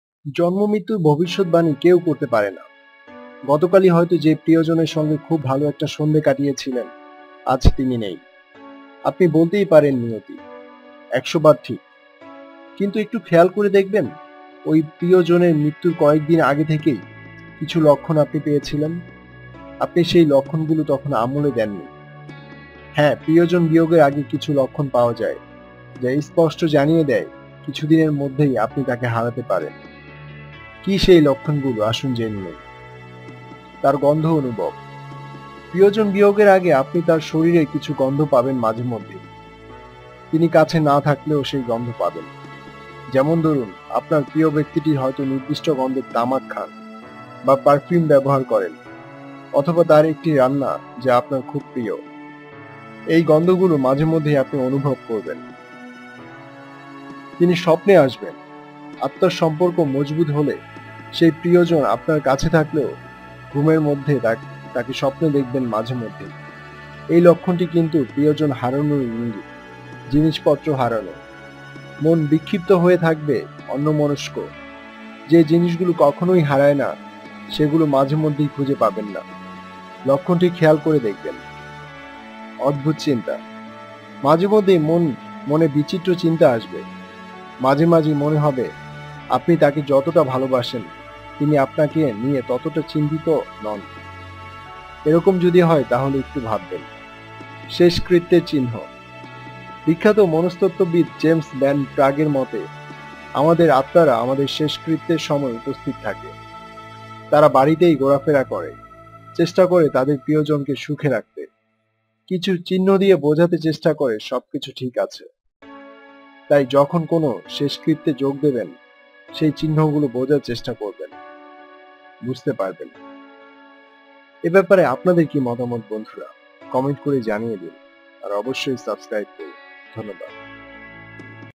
৫ চিহ্নে বুঝবেন, আপনার কাছের জন মারা যাবেন
জন্ম-মৃত্যুর ভবিষ্যদ্বাণী কেউই করতে পারে না। গতকালই হয়তো যে প্রিয়জনের সঙ্গে খুব ভালো একটা সন্ধে কাটিয়েছিলেন, আজ তিনি নেই। আপনি বলতেই পারেন নিয়তি। একশোবার ঠিক। কিন্তু একটু খেয়াল করে দেখবেন, ওই প্রিয়জনের মৃত্যুর কয়েক দিন আগে থেকে কিছু লক্ষণ আপনি পেয়েছিলেন। আপনি সেই লক্ষণগুলি আমল দেননি। হ্যাঁ, প্রিয়জন বিয়োগের আগে কিছু লক্ষণ পাওয়া যায়, যা স্পষ্ট জানিয়ে দেয়, কিছুদিনের মধ্যেই আপনি তাঁকে হারাতে পারেন।
কী সেই লক্ষণগুলি?